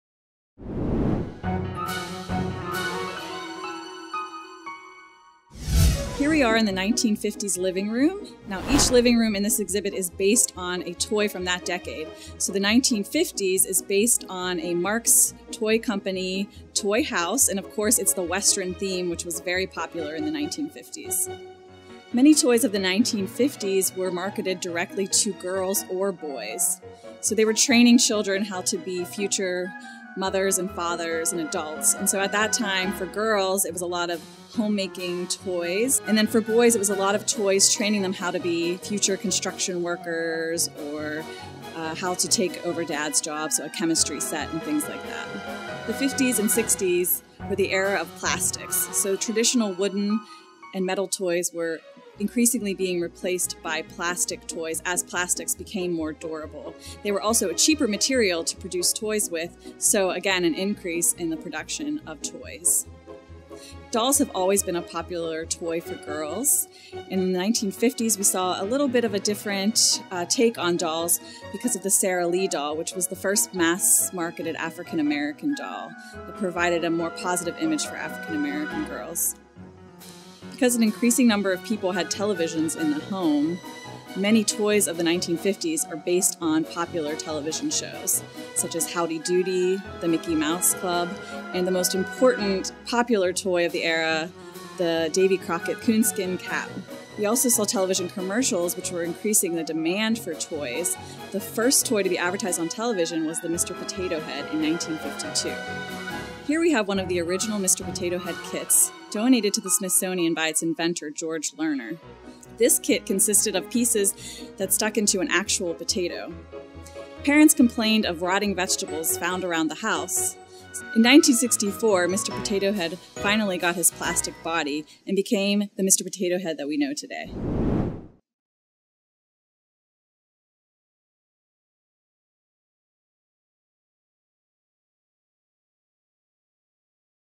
1950s | Virtual Tour | Toys of the '50s, '60s and '70s
Take a virtual tour of the History Center's Toys of the '50s, '60s and '70s exhibit. This stop - the 1950s! Learn more about popular 1950s toys such as Mr. Potato Head, the Sara Lee doll, and the Davy Crockett coonskin cap.